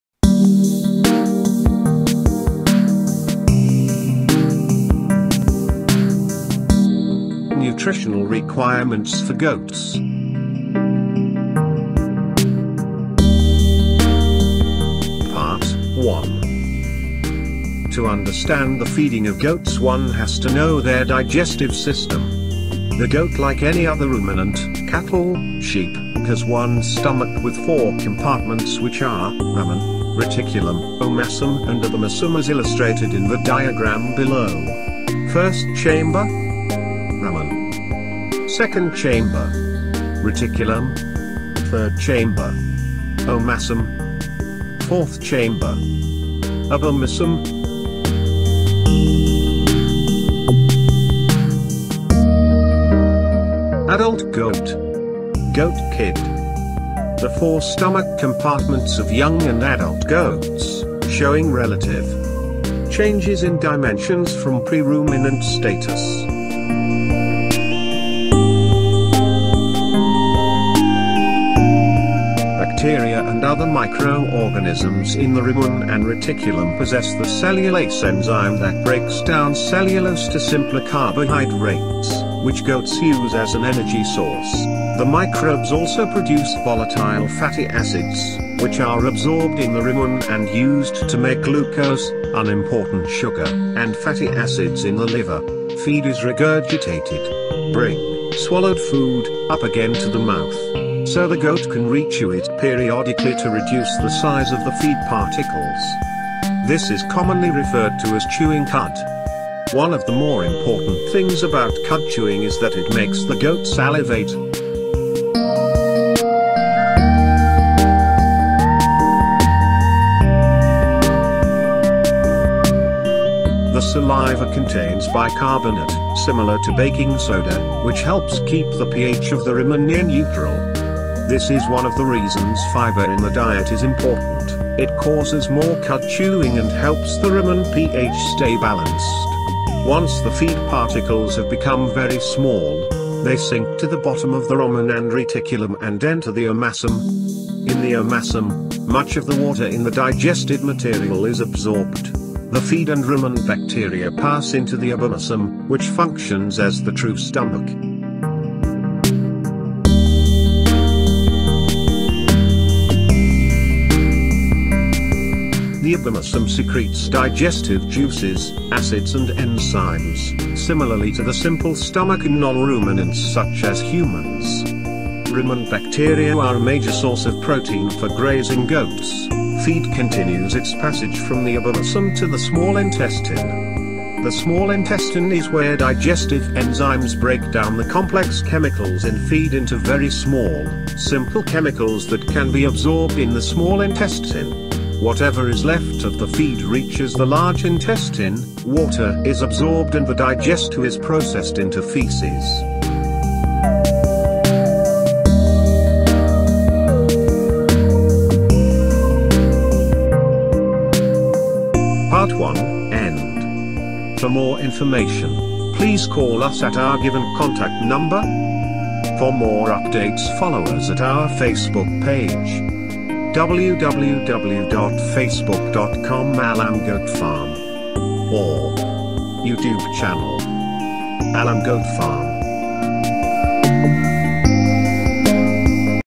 Nutritional Requirements for Goats Part 1
Feed cost account for a higher proportion of the operating cost. It is therefore important that great attention should be paid to the principles of nutrition and feeding in order to optimize productivity and profits from goat production.